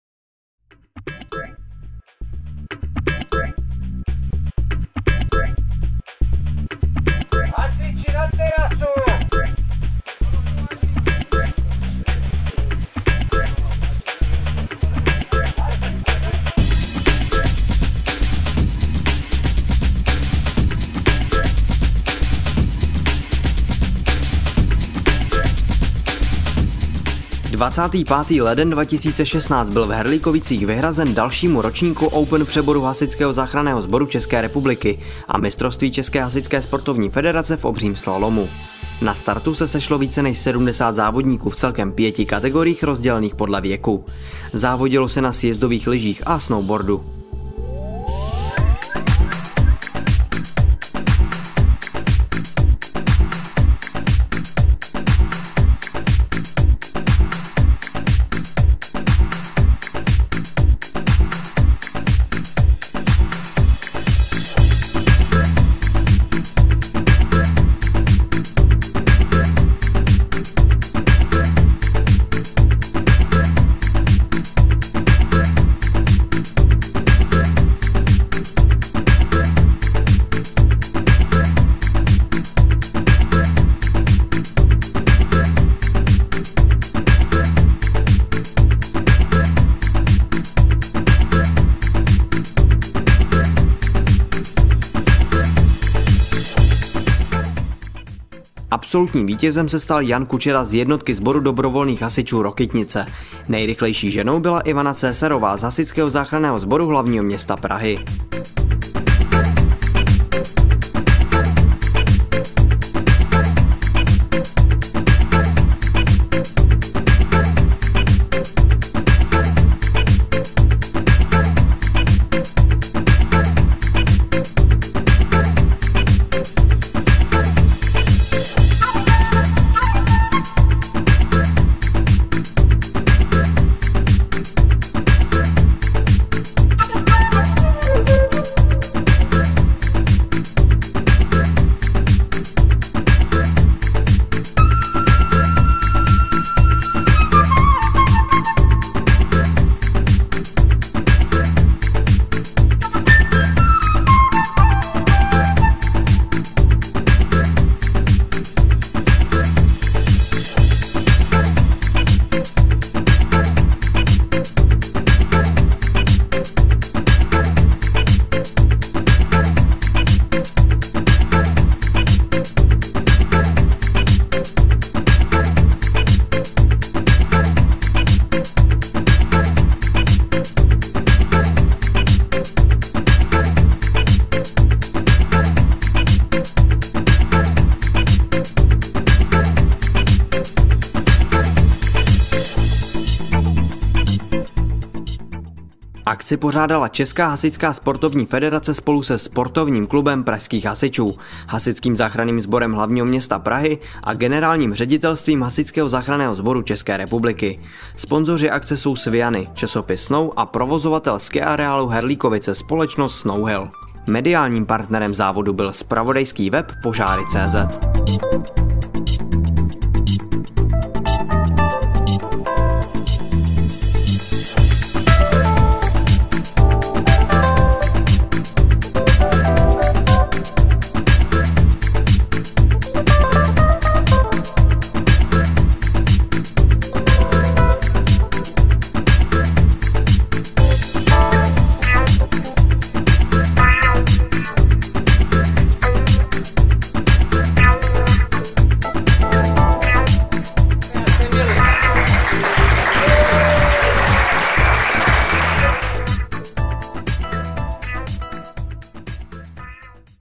POŽÁRY.cz: Mistrovství ČHSF v obřím slalomu 2016 vyhrál dobrovolný hasič Jan Kučera
25. leden 2016 byl v Herlíkovicích vyhrazen dalšímu ročníku Open přeboru HZS ČR a mistrovství České hasičské sportovní federaci v obřím slalomu. Na startu se sešlo více než 70 závodníků v celkem pěti kategoriích rozdělených podle věku.  Závodilo se na sjezdových lyžích a snowboardu.